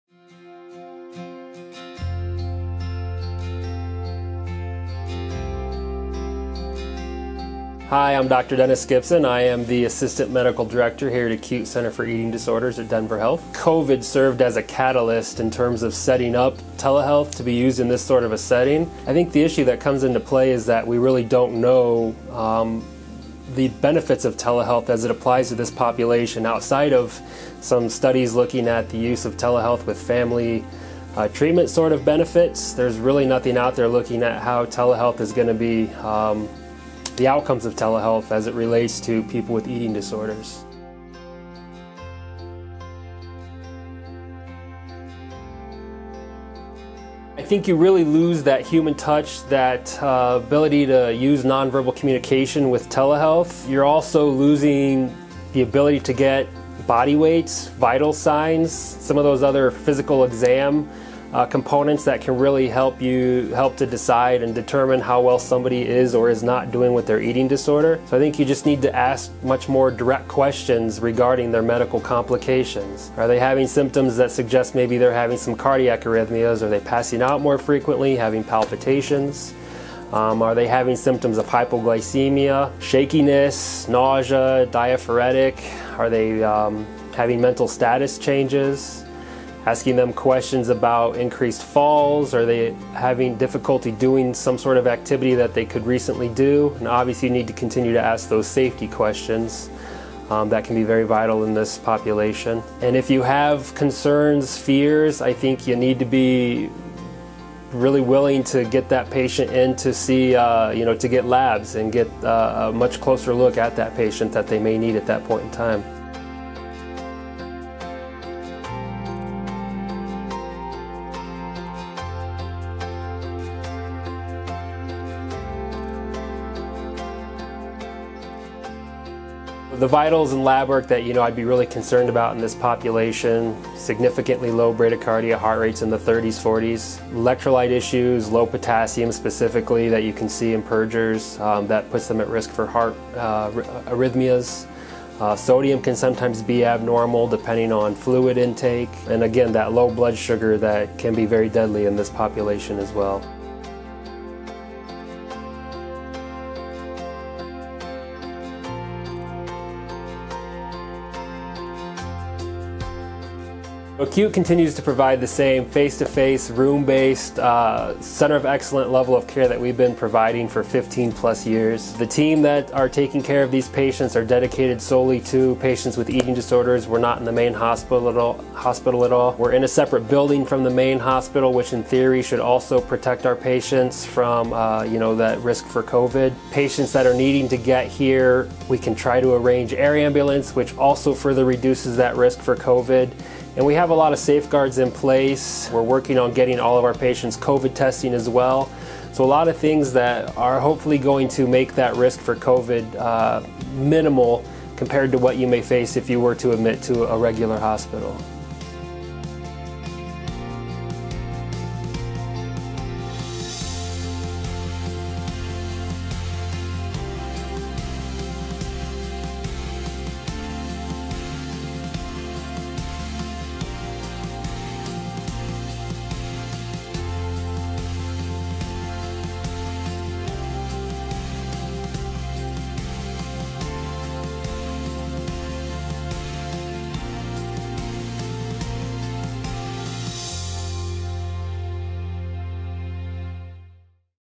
COVID-19, Telehealth and Identifying Medical Complications of Severe Eating Disorders
Dennis Gibson, MD, CEDS, Assistant Medical Director of the ACUTE Center for Eating Disorders at Denver Health, provides guidance to primary care physicians and other family medicine practitioners using telehealth as a bridge for patient care during COVID-19. Because the pandemic may be triggering to patient with eating disorders, providers must be vigilant to inquire about signs of medical decline in patients with severe eating disorders and collect labs and vitals urgently. Untreated medical complications of severe eating disorders likely pose greater risk than COVID-19 infection, and medical providers can be a valuable advocate for their patients in this uncertain time.